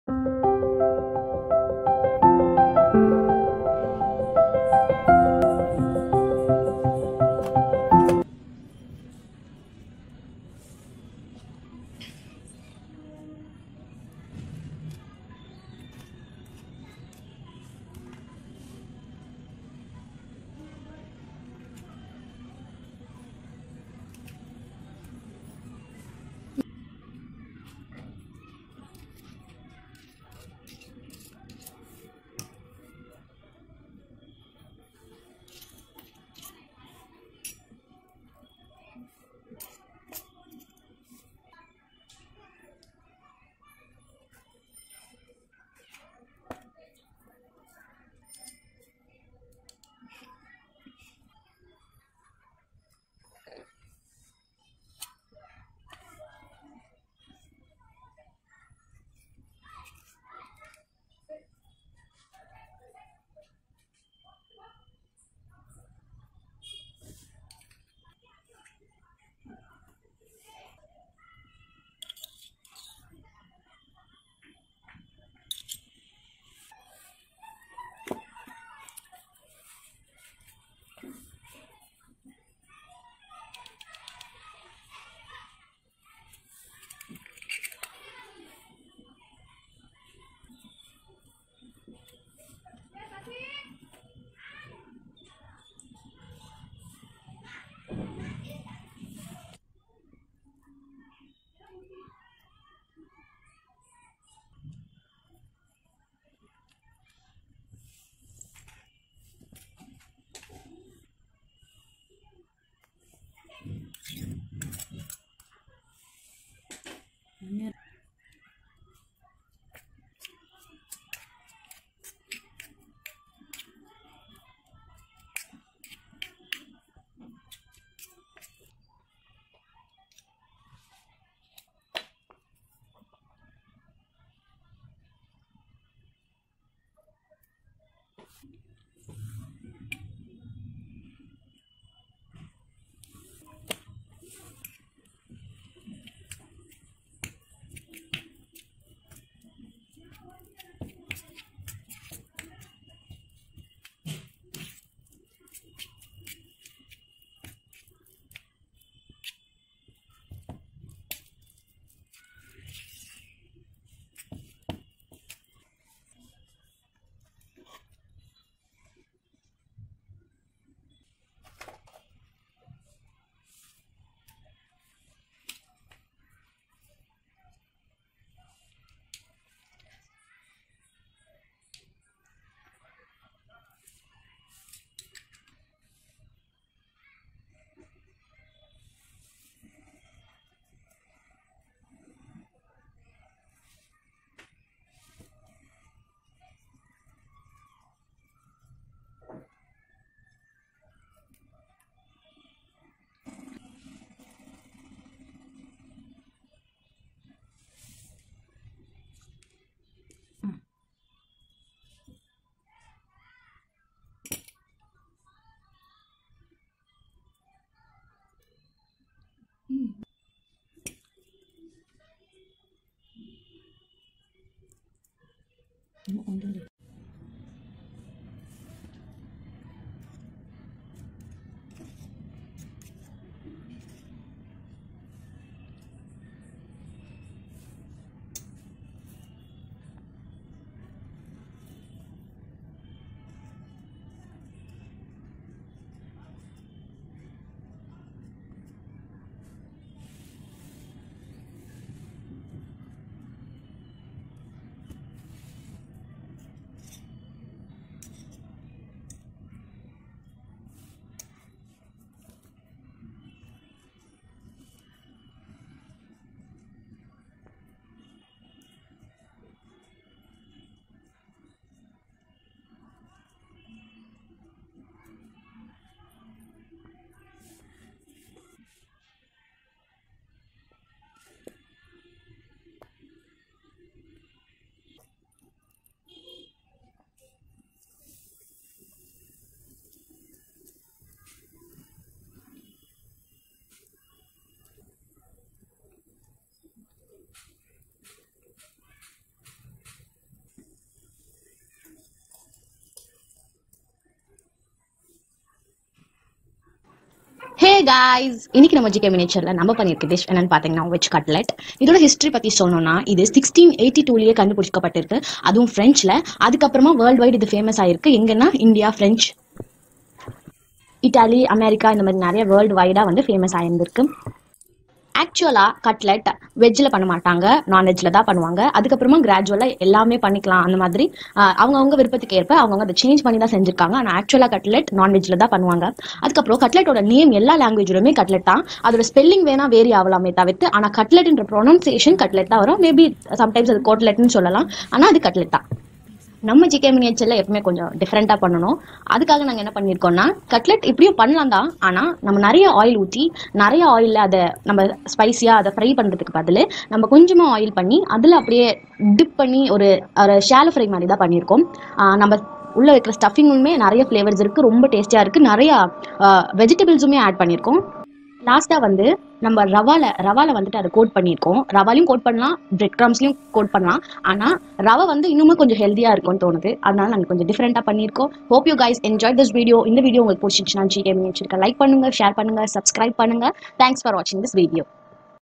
Veg Cutlet Recipe 😎 ||GK Miniature Cooking👩‍🍳 || #18 || Tamil
GK Miniature is an interesting video on Miniature Cooking.
In this video we have tried a Different Cutlet Recipe.
This video also helps to know the history of the dish and also some facts of it.

This Miniature Kitchen is full and Full made up of cardboard and all the furniture like  cupboard , Fridge, Sink everything is Handmade.

Camera & Editing By :   Gokula Priya J
Cooking and History  By :  Krishna Priya J